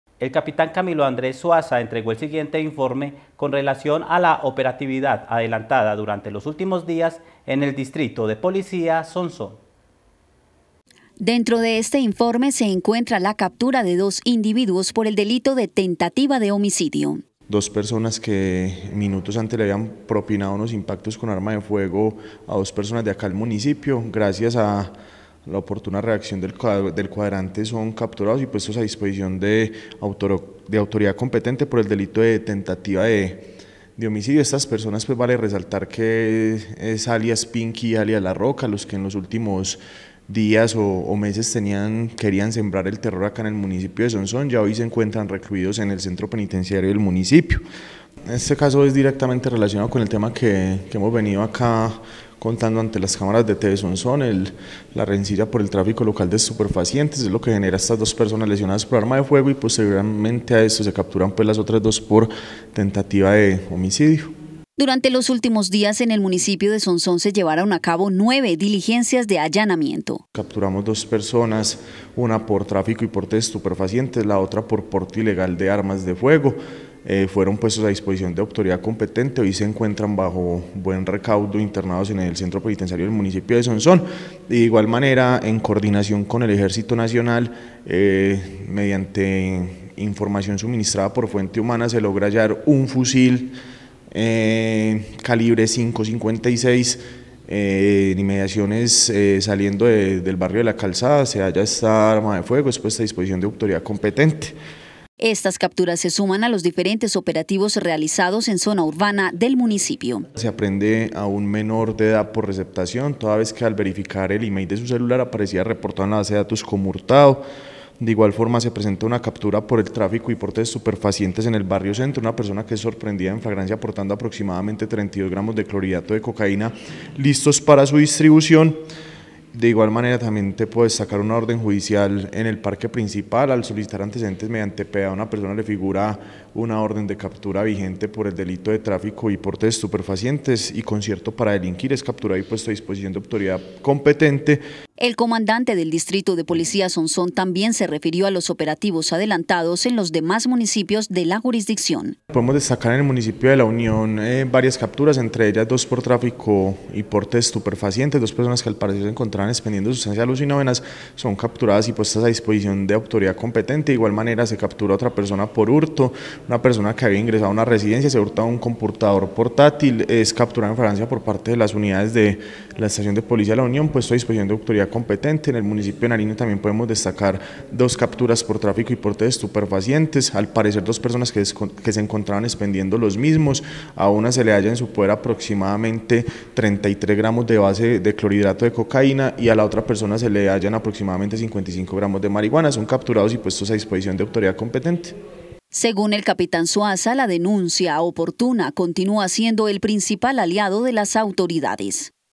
OPERATIVIDAD EN EL DISTRITO DE POLICÍA SONSÓN
El capitán Camilo Andrés Suaza entregó el siguiente informe con relación a la operatividad adelantada durante los últimos días en el distrito de policía Sonsón.